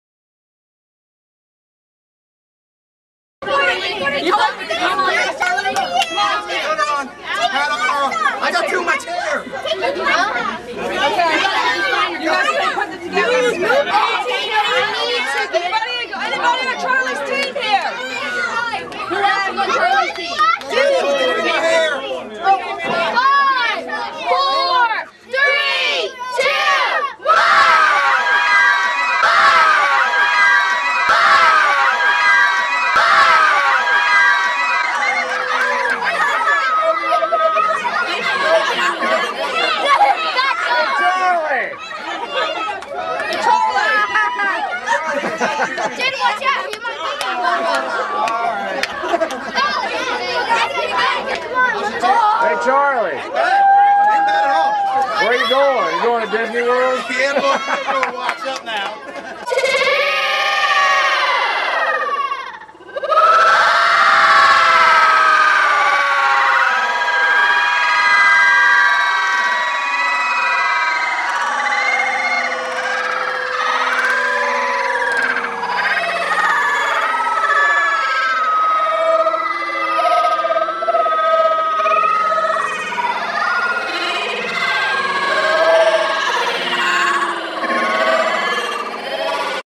Charlie Cook Pie In The Face
This is Charlie Cook getting a pie in the face from some players from his football team.